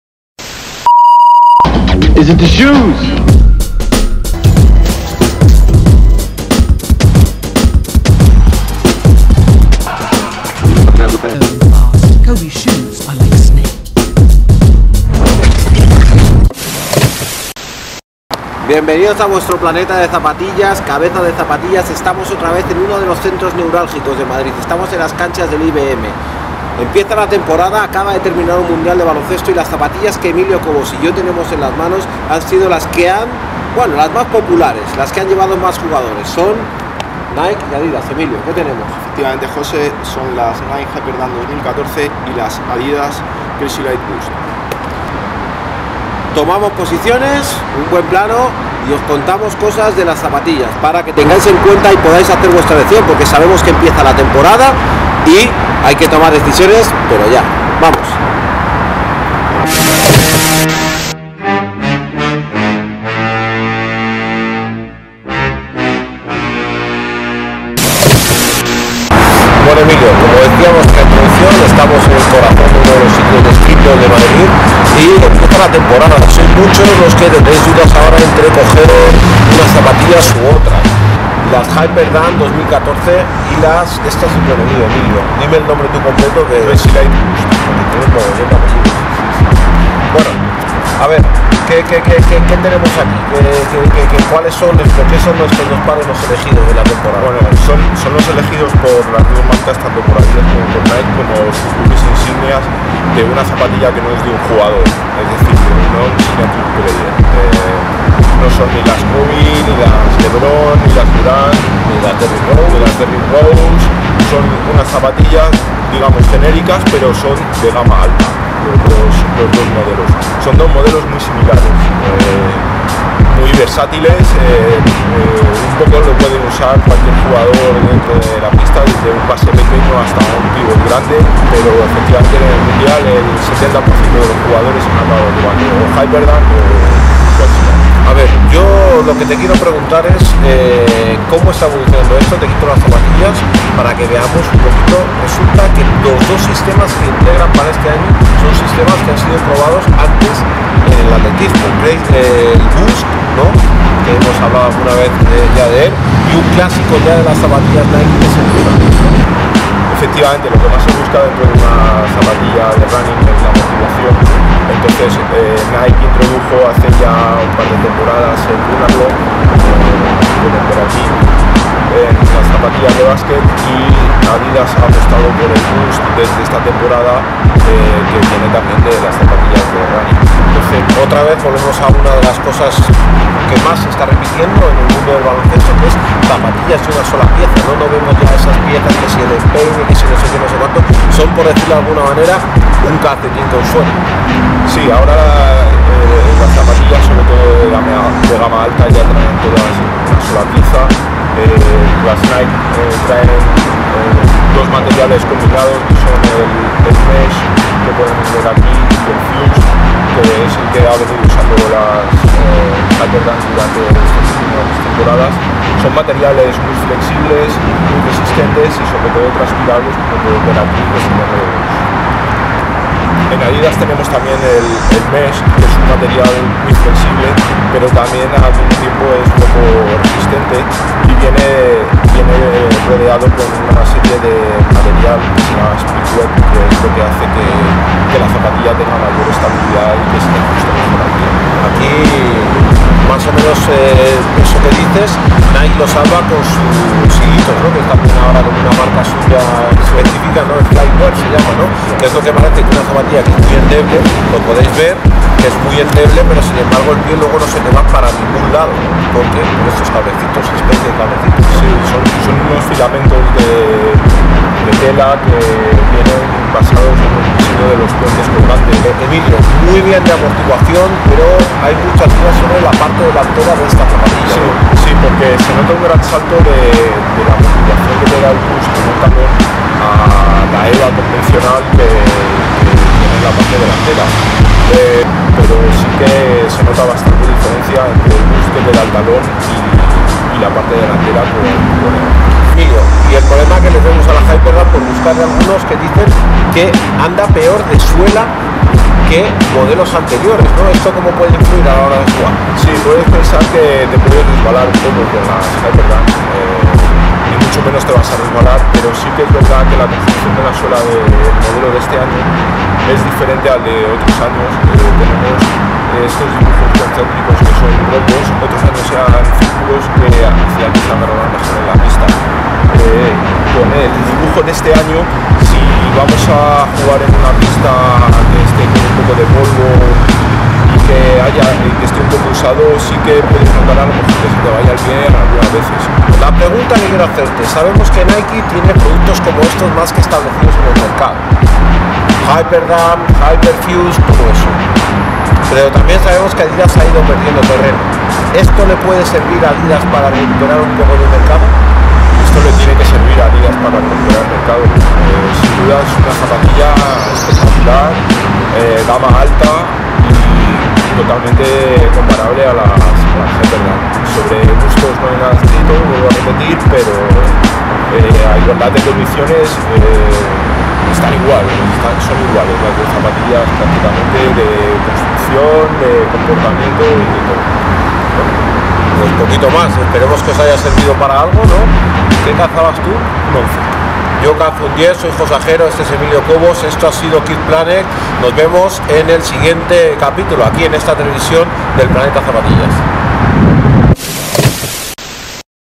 KickPlanet TV. Ep.1 HyperDunk vs.CrazyLight Boost
¿A quién quieres más? ¿a Papá? ¿a Mamá? Te enseñamos los dos grandes caballos ganadores de Adidas y Nike para el Mundial de España 2014... y sobre todo, para la temporada que empieza. Escoge tu arma.